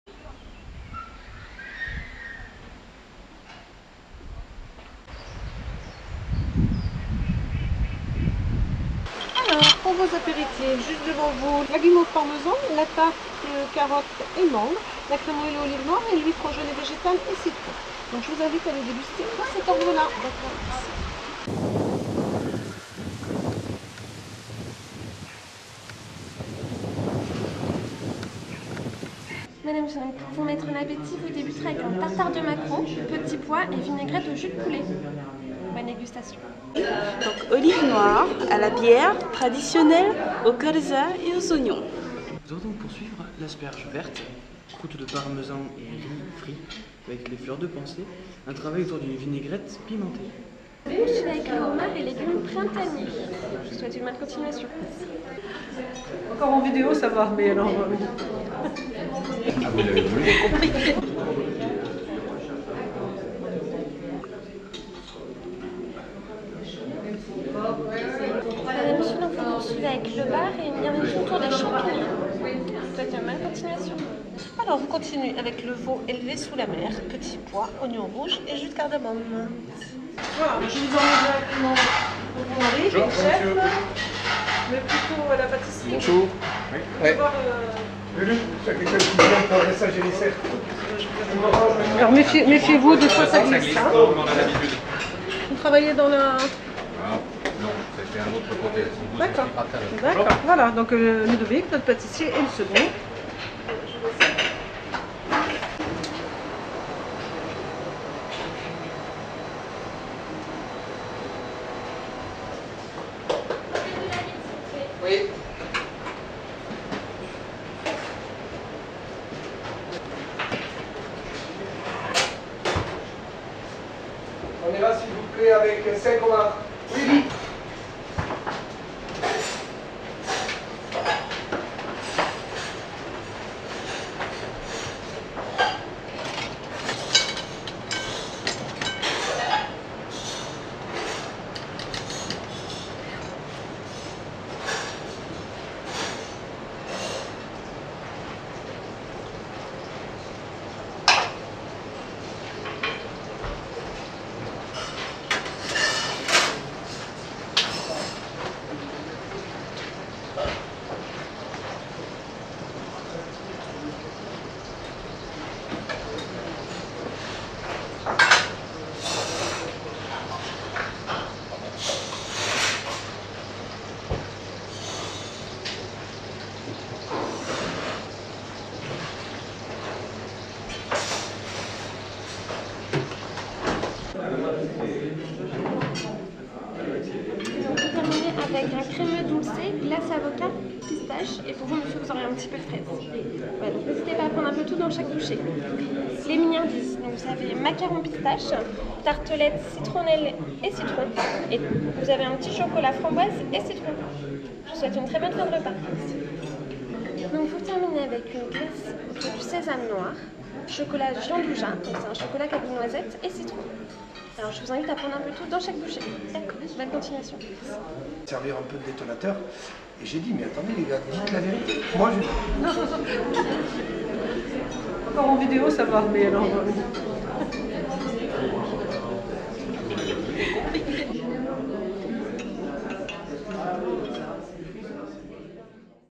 Déjeuner à Passions & Gourmandises * - 15 juin 2013
Après avoir officié pendant près de 15 ans dans leur "Champ de Foire" de Lencloître, établissement distingué dès 1996 d'un R (devenu Bib Gourmand en 97), Laure & Richard Toix se sont installés à Saint-Benoît, charmant petit village de presque 7 000 âmes où ils ont repris en mai 2007 le "Chalet de Venise" transformé en "Passions et Gourmandises". Dès l'édition 2008 de la petite bible rouge de l'hexagone culinaire, l'étoile est venue récompenser les efforts déployés par ce couple et son équipe (à ce sujet, le trombinoscope du site n'est pas à jour, dommage). Situé en bordure du Miosson, petite rivière d'une trentaine de kilomètres, l'établissement, à la structure moderne, est accueillant et reposant.